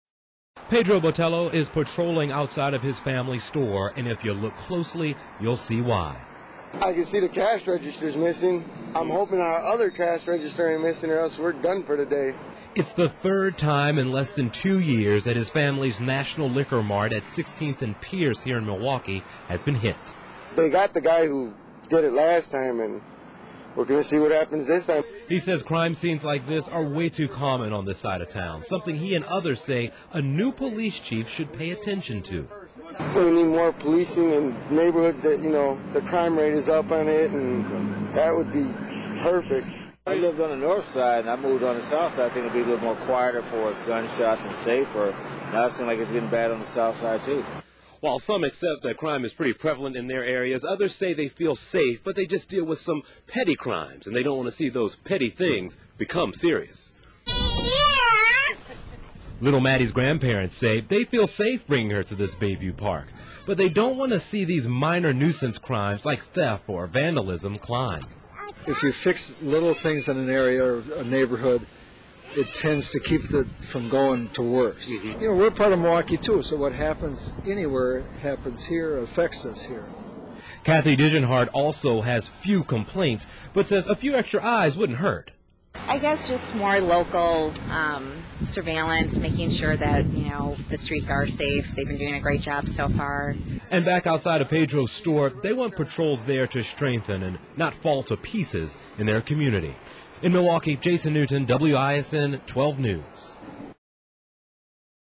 Citizens Concerned That Small Crimes Will Get Worse
Citizens across Milwaukee have growing concern that petty crimes will get worse.  A few talked with WISN 12's Jason Newton about what they want in a new top cop.